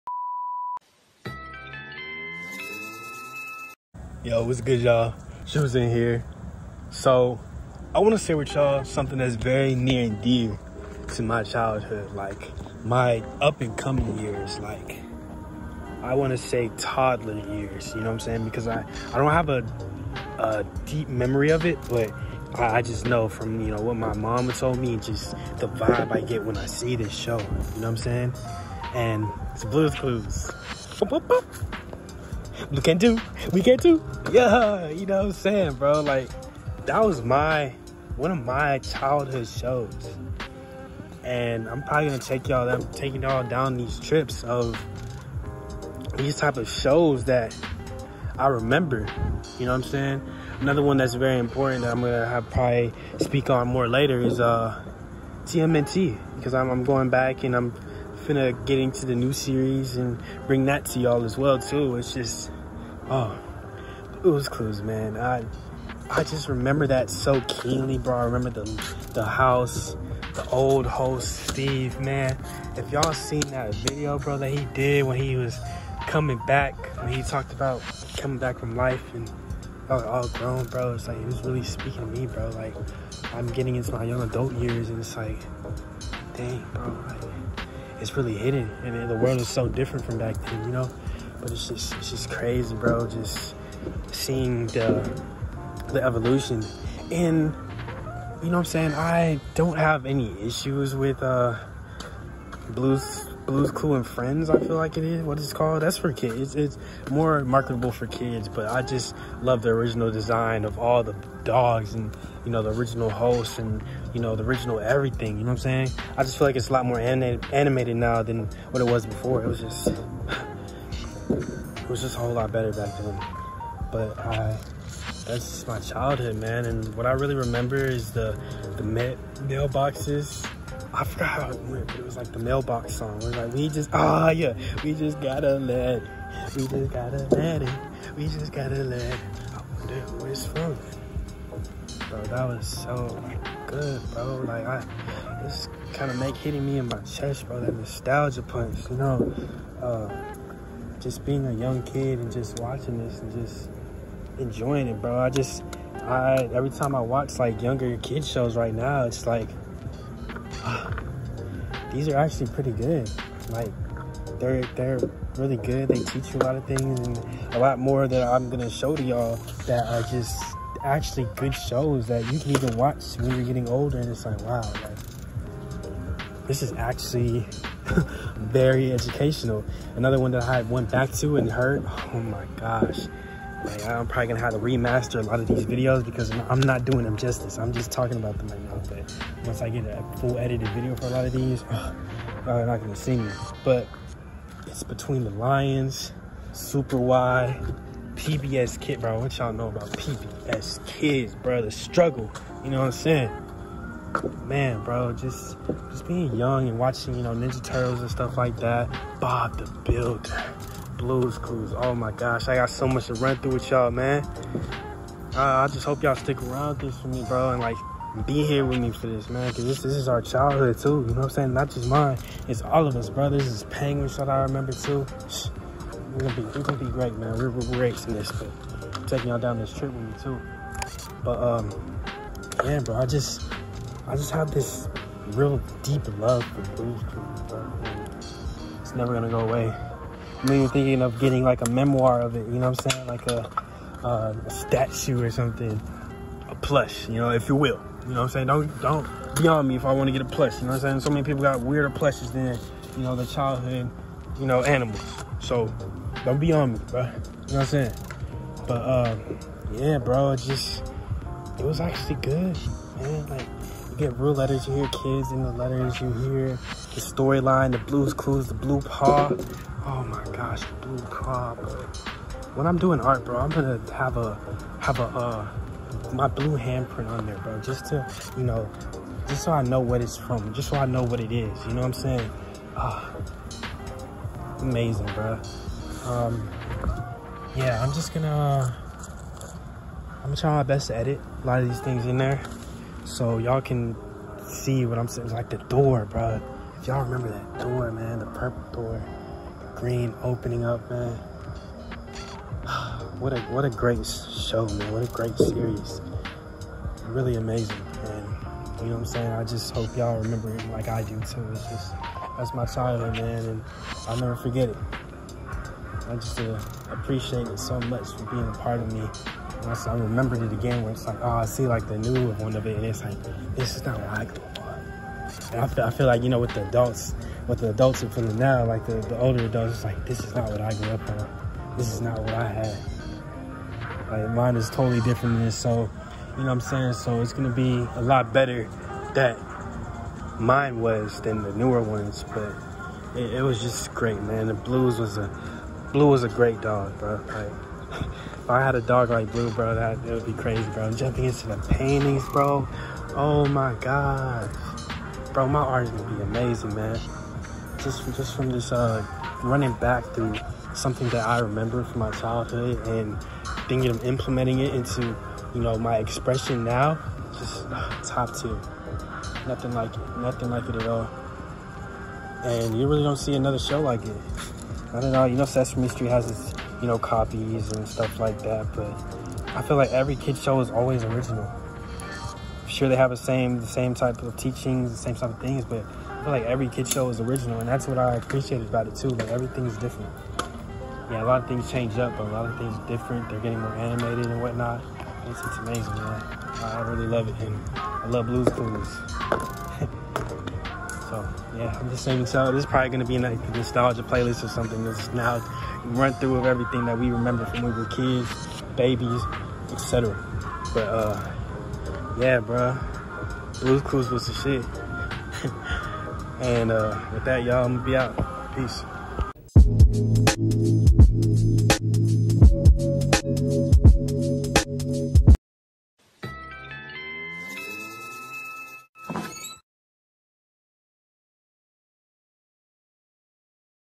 Blue’s Clues is One of The BEST Children’s Shows EVER!
Yo, thanks for checking out the video!
In this video I touched on:

My Nostalgia Trip with Old Blues Clues. Acknowledging the newer one for its changes. Remembering Steve’s message to us now young adults. Stating this as One of BEST the Children’s Shows Ever.

The Socials